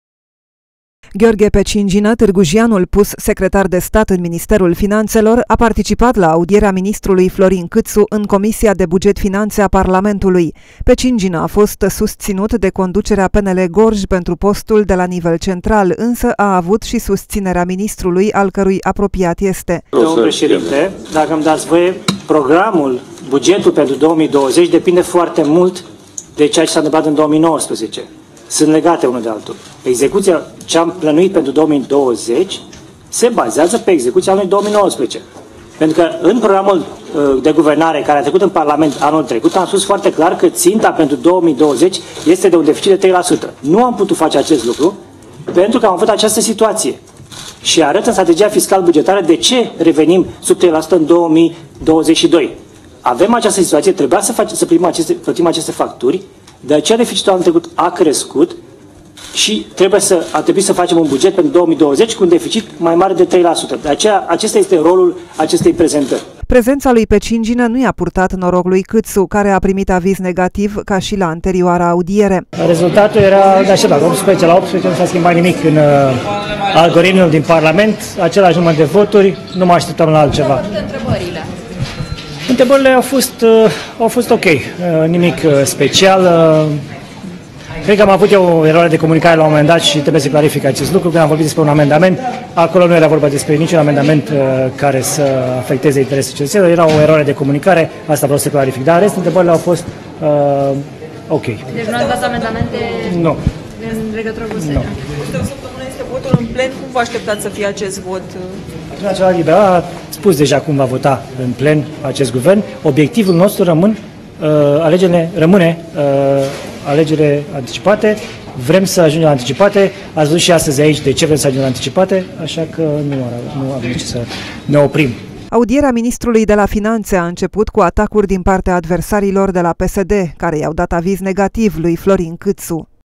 PECINGINĂ L A ÎNSOȚIT PE CÂȚU LA AUDIERI
Secretarul de stat gorjean, Gheorghe Pecingina, l-a insotit pe ministrul Finantelor, la audierile din Parlament. Liberalul de la Targu Jiu, promovat in minister, este unul dintre apropiatii lui Catu